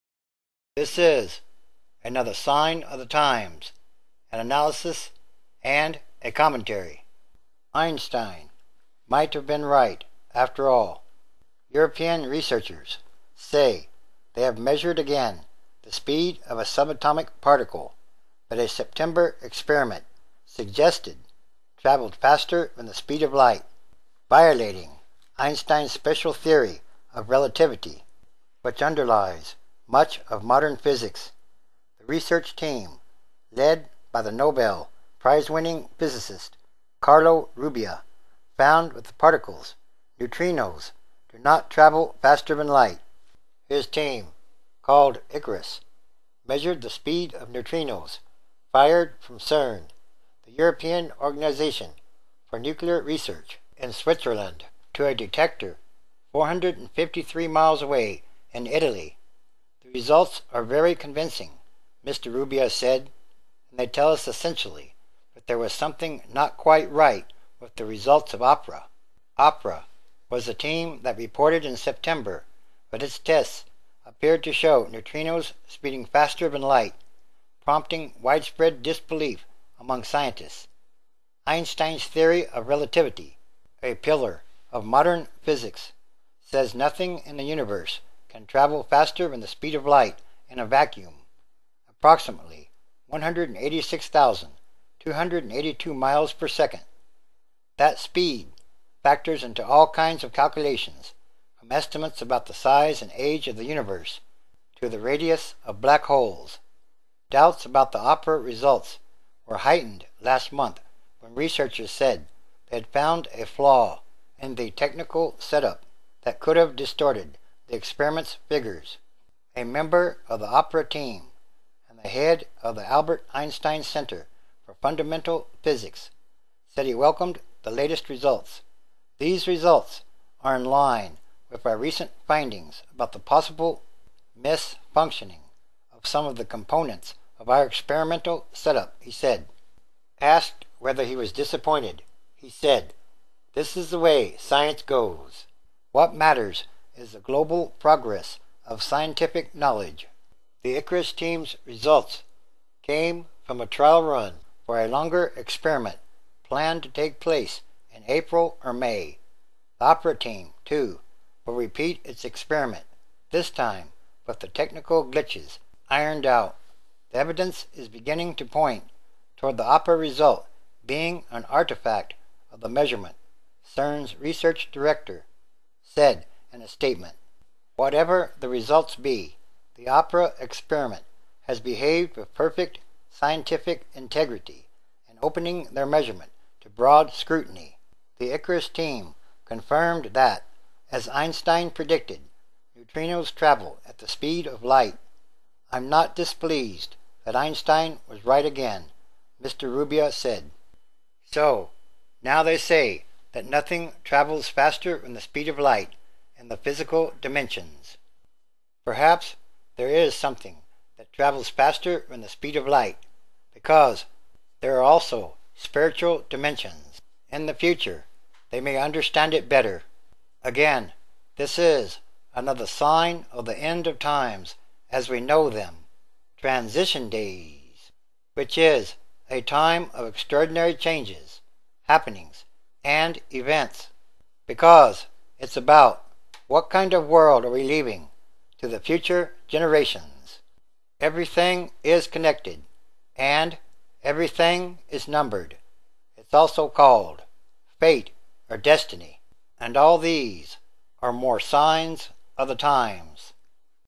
Neutrinos Not Faster Than Light In Retest, Scientists Say
European researchers now say that Einstein might have been right after all. Researchers have measured the speed of a subatomic particle that a September experiment suggested traveled faster than the speed of light, violating Einstein's special theory of relativity, which underlies much of modern physics.